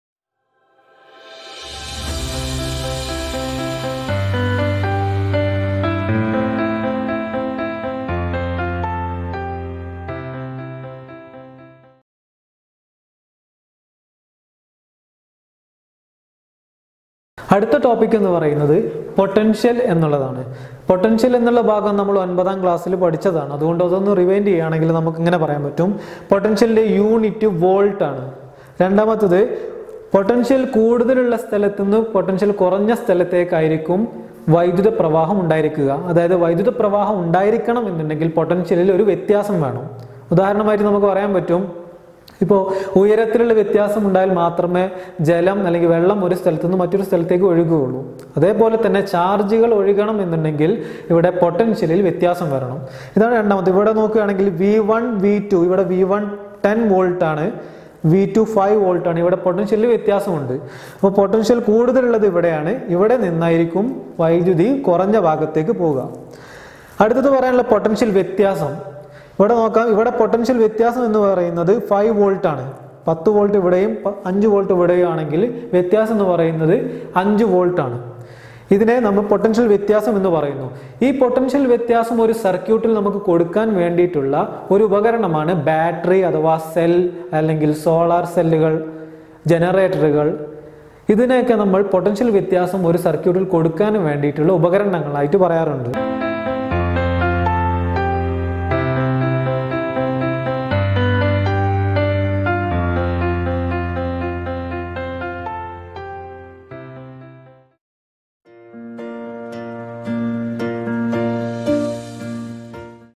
SSLC physics- Chapter 1| Topic 2 Potential|Effects of electric current
10 th standard physics class/ easy exam cracker/SSLC chapter 1/Topic 2 Potential difference/
kerala syllabus online class/
physics crash/physics SSLC/
This is the third part of Kerala State syllabus physics class/ Effects of electric current/ more classes, revisions, question paper solving and crash classes for revising important points also will be followed soon